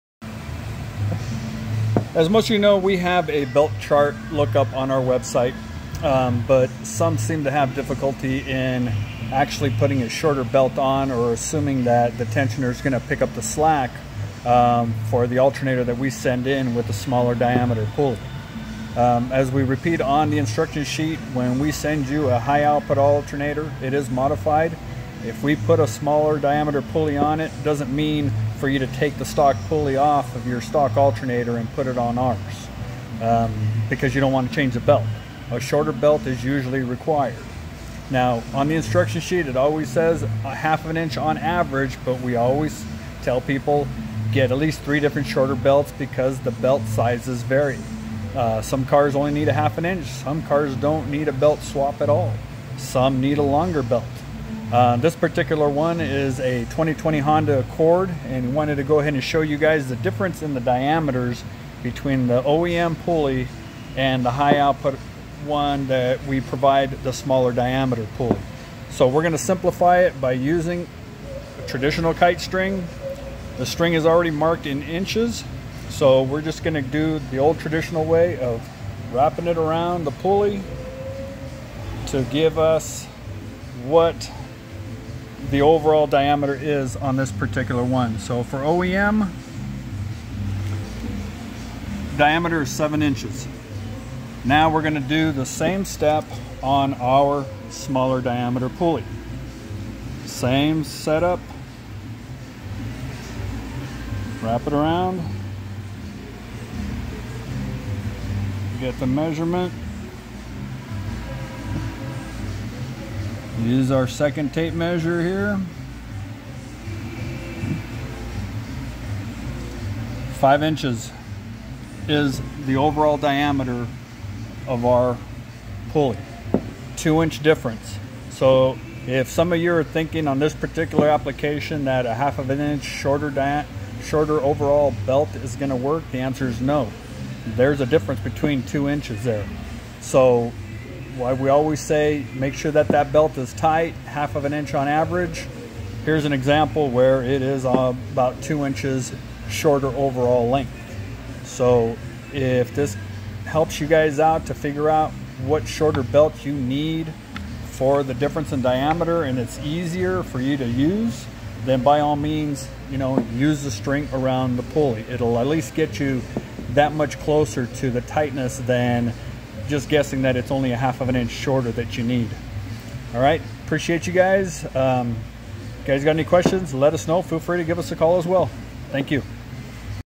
Finding the proper belt size by comparing pulley diameters.
We have a belt lookup chart on our website,  but this simplifies how much of a size you will need to look for.